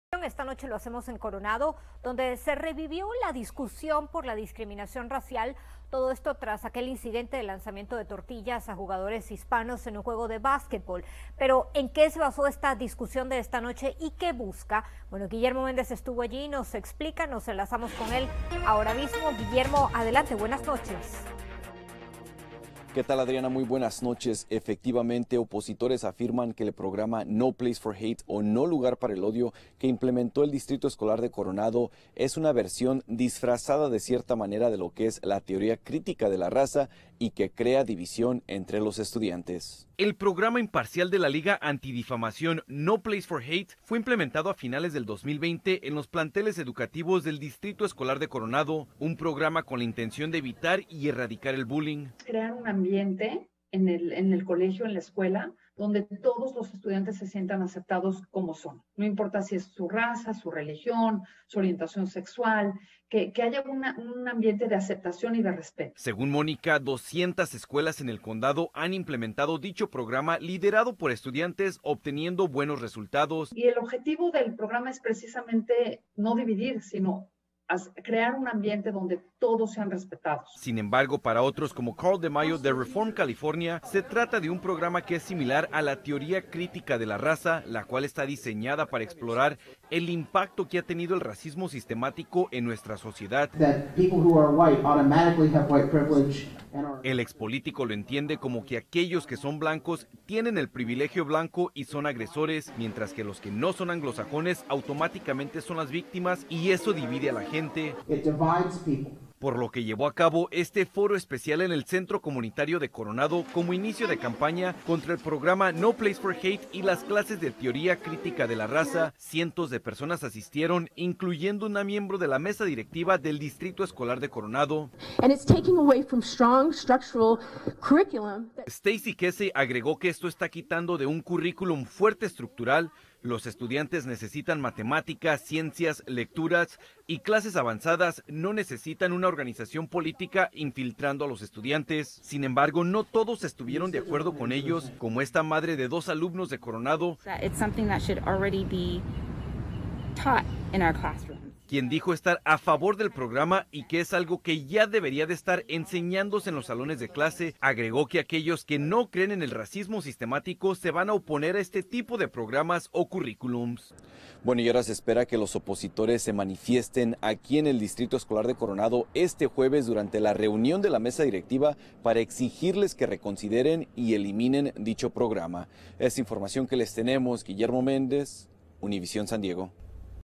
Buscan eliminar programa anti-odio del Distrito Escolar de Coronado
"No Place for Hate" busca eliminar el bullying en las escuelas, pero opositores afirman que es un programa disfrazado del aprendizaje de la "Teoría Crítica de la Raza".